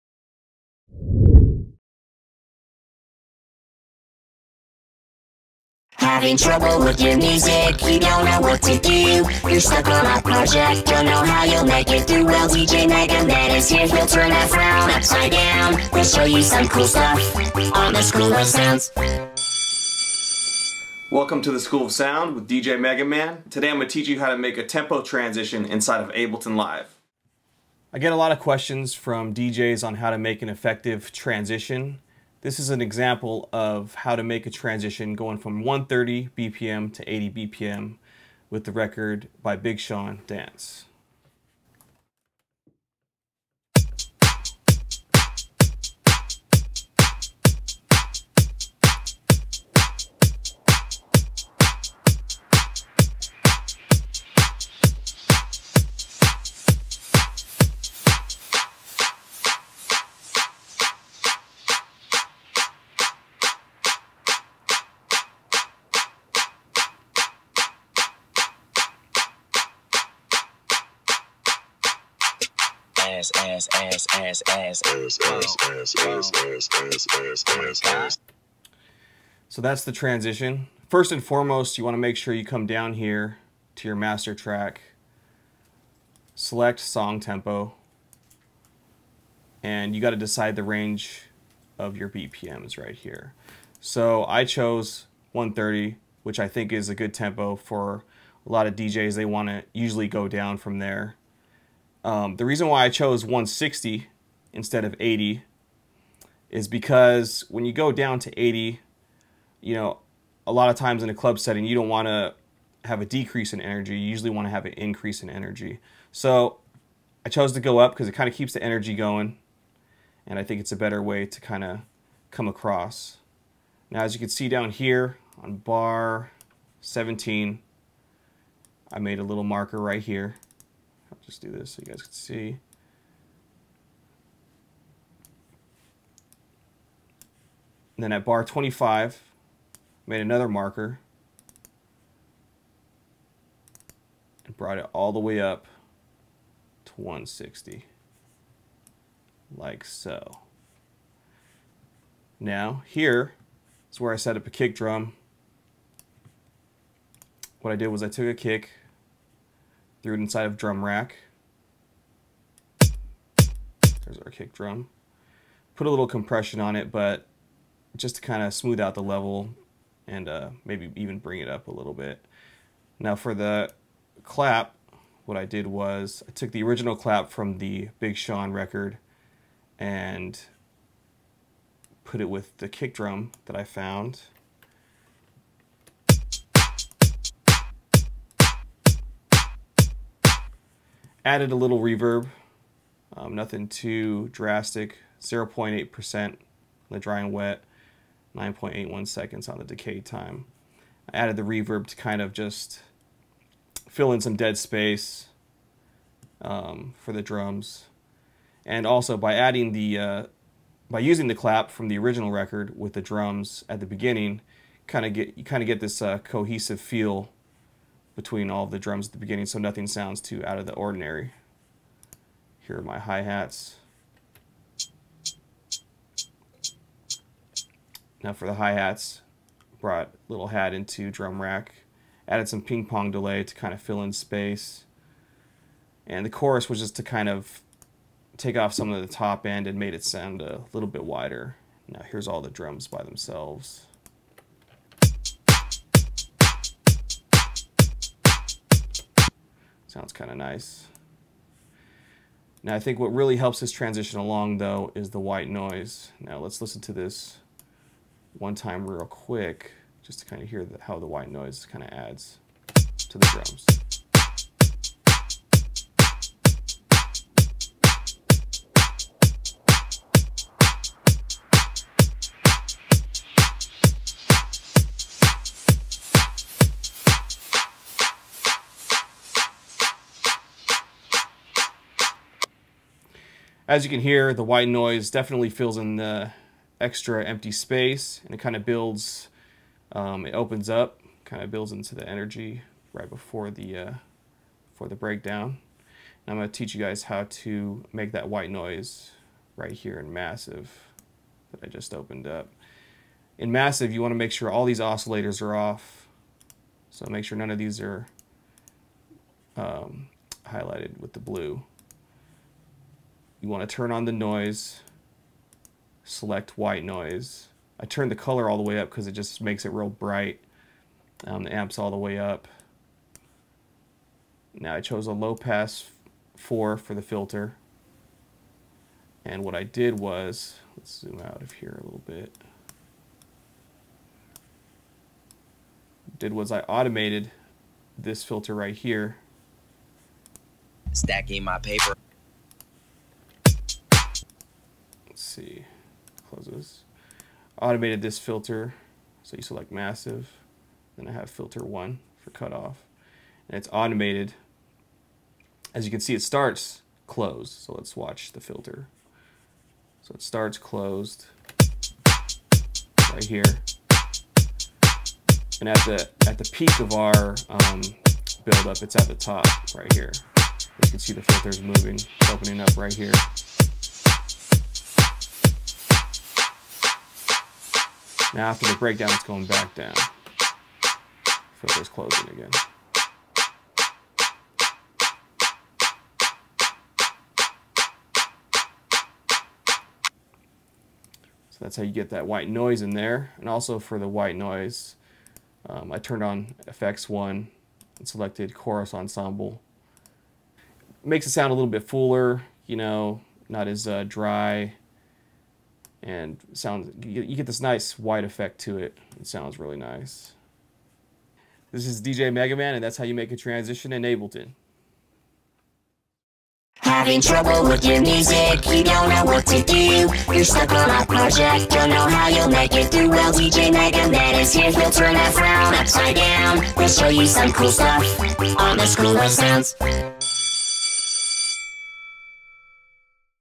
School of Sounds: How to Make a Tempo Transition in Ableton Live
DJ Mega Man explains how to make a simple tempo transition with ease in Ableton Live.

To download The School Of Sounds: Tempo Transition Ableton Session,